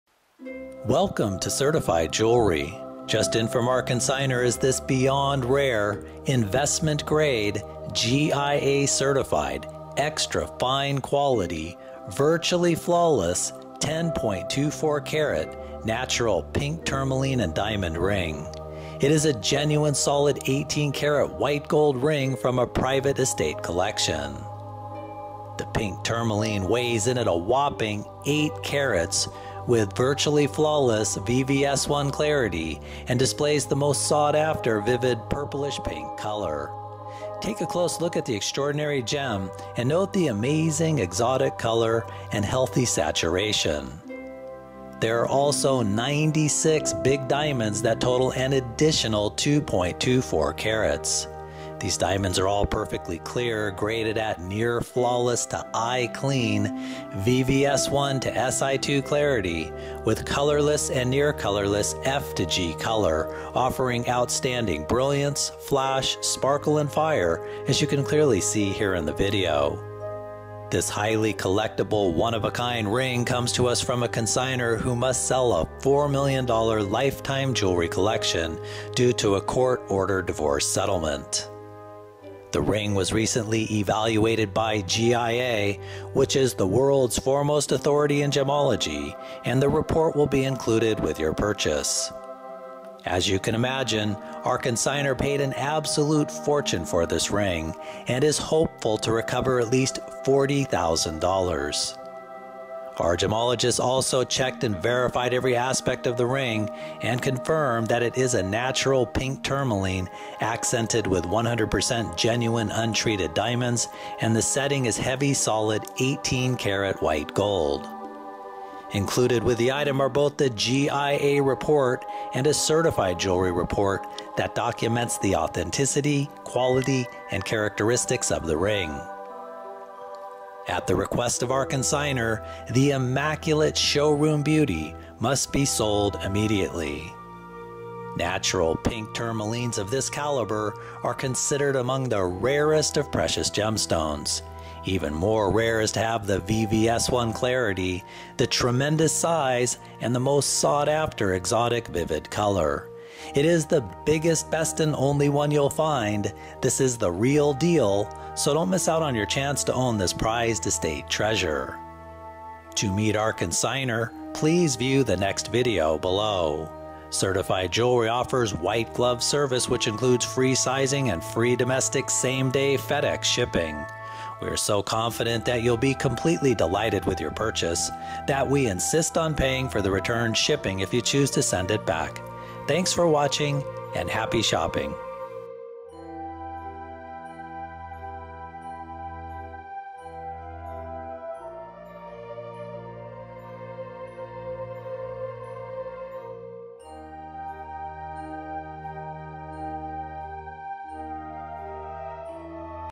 GIA Certified VVS1 Natural Pink Tourmaline Diamond 18k White Gold Estate Ring GEM - C552
PRIME Quality Investment Grade VIVID Color Gem SEE VIDEO

Immaculate Condition - Top Gem "Absolutely Huge" 10.24 Carats GIA Certified Natural Pink Tourmaline & Diamond Cocktail Ring. This dazzling pink tourmaline specimen is a jaw dropping "8.00 carats" with near flawless VVS1 clarity and a rich Purplish Pink Color found only in the finest of genuine Brazilian tourmaline gemstones. There is also a generous 2.24 carats of absolutely gorgeous F-G color round brilliant colorless & near colorless VVS/SI accent diamonds that will light up the room with sparkle and fire. The setting is crafted in heavy solid 18k white gold and clearly the work of a true jewelry master craftsman. This GIA Certified Estate Beauty has it all including a heavy classic custom made solid 18k white gold setting. A Rare Stunning Beauty. Don't let this one get away!